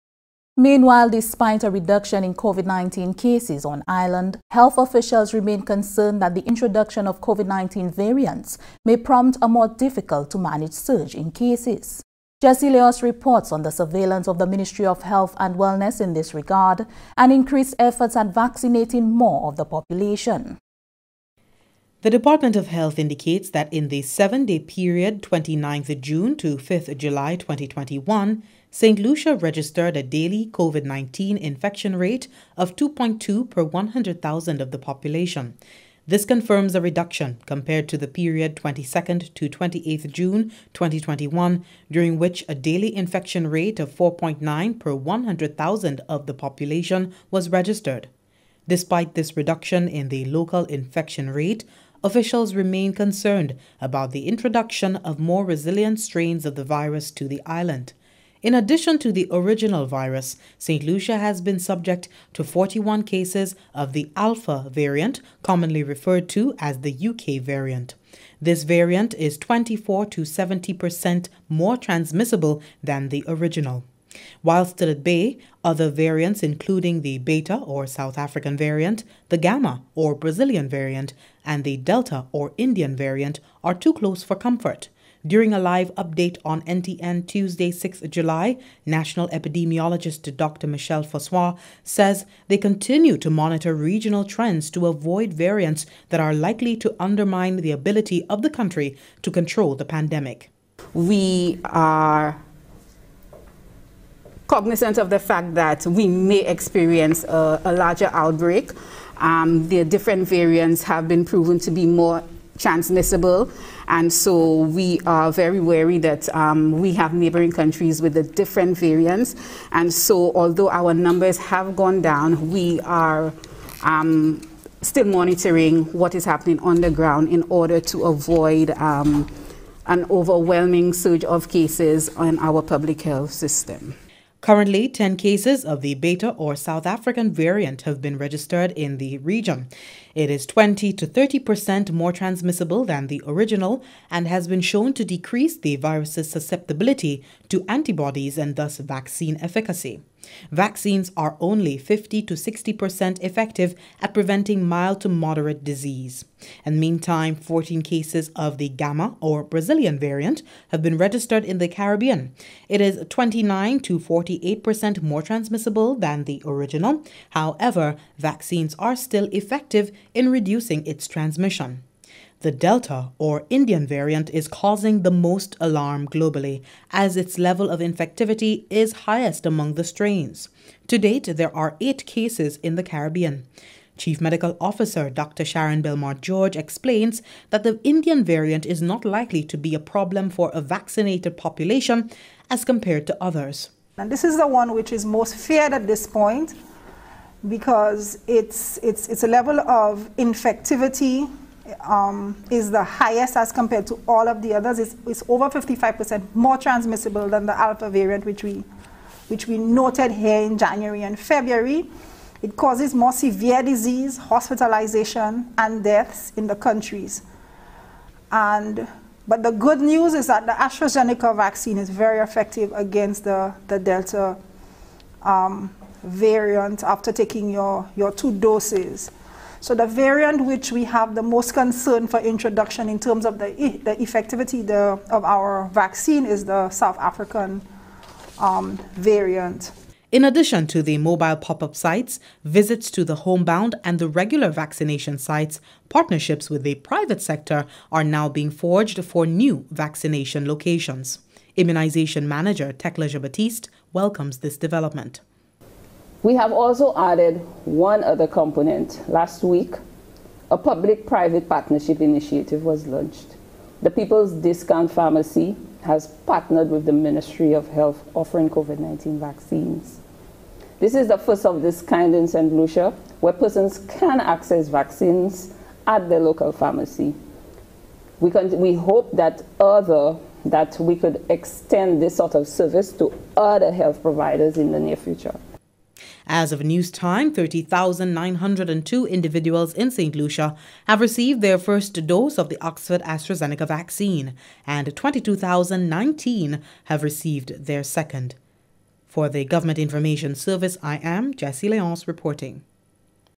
Covid19 Variants Concerns
Despite a reduction in cases on the island, health officials remain concerned that the introduction of Covid19 variants may prompt a more difficult to manage surge in COVID19 cases.